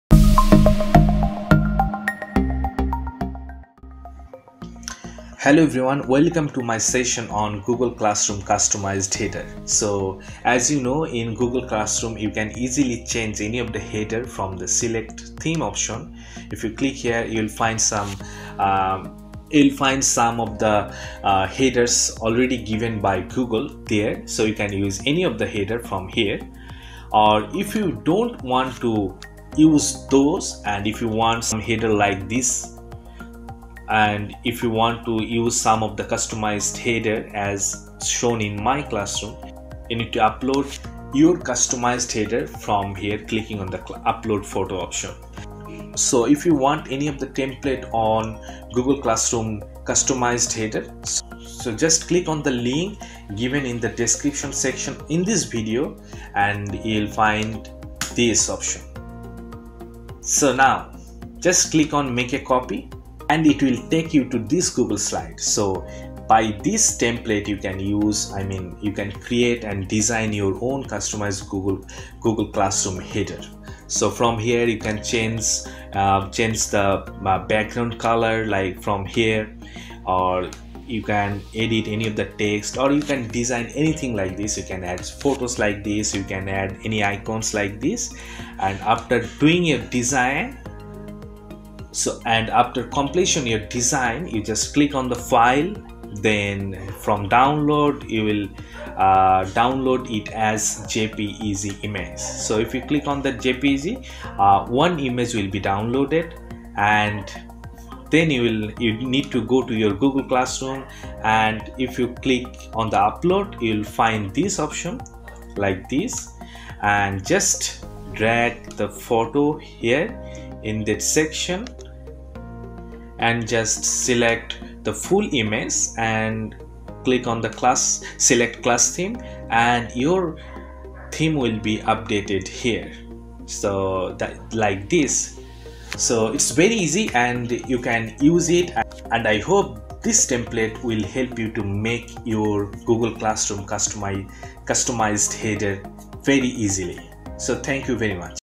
Google Classroom Custom Header- Design your Own (Super Easy)
💻 See how you can design and use your own customized Google Classroom Header.